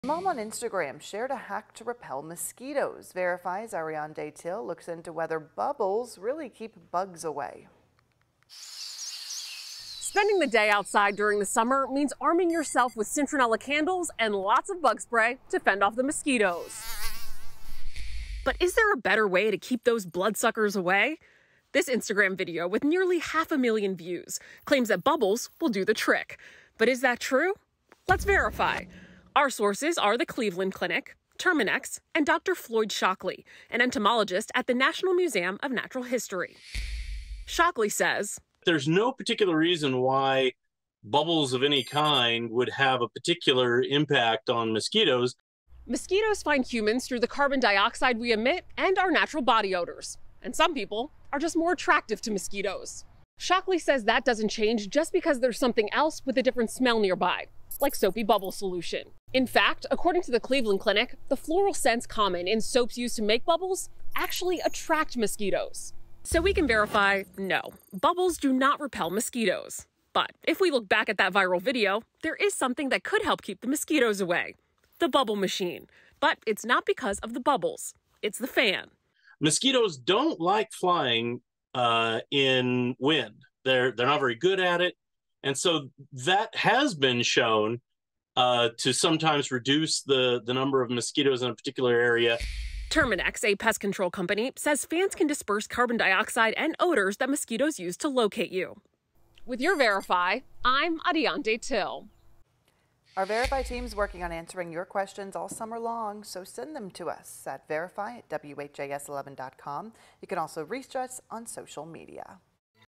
Verify: No, bubbles do not repel mosquitoes
Mosquitoes find humans through a combination of the carbon dioxide we emit and our normal body odors.